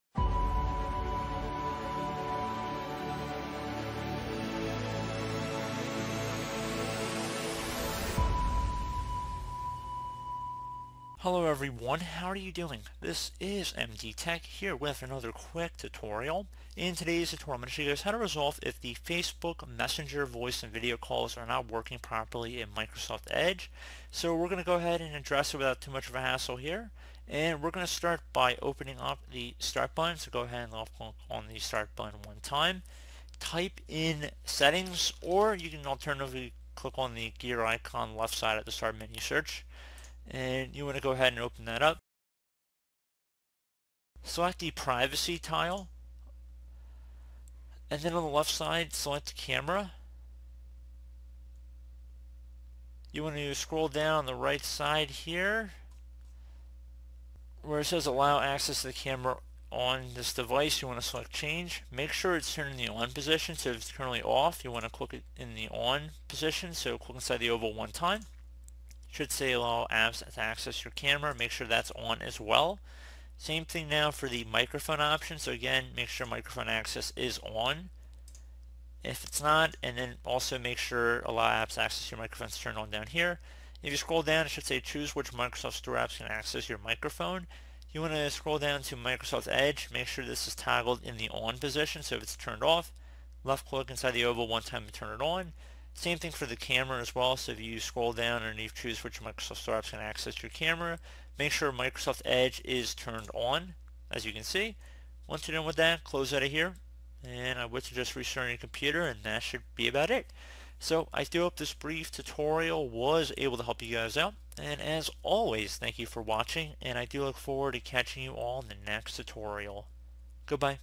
Facebook Messenger Voice and Video Calls Not Working with on Microsoft Edge FIX [Tutorial]
Facebook quietly upgraded its Messenger app for Windows 10 with the ability to make voice and video calls. The feature worked with all major browsers like Google Chrome, Mozilla Firefox, and Microsoft Edge. However, many users report the Facebook Messenger voice calls and video calls do not work on Microsoft Edge as advertised. In this tutorial, we look at this issue and try to fix it.